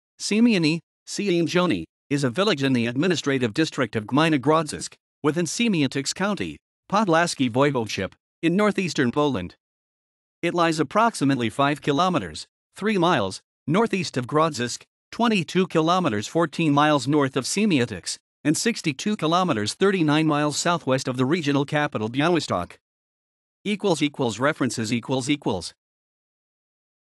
Siemiony
Siemiony [ɕeˈmjɔnɨ] is a village in the administrative district of Gmina Grodzisk, within Siemiatycze County, Podlaskie Voivodeship, in north-eastern Poland. It lies approximately 5 kilometres (3 mi) north-east of Grodzisk, 22 km (14 mi) north of Siemiatycze, and 62 km (39 mi) south-west of the regional capital Białystok.

== References ==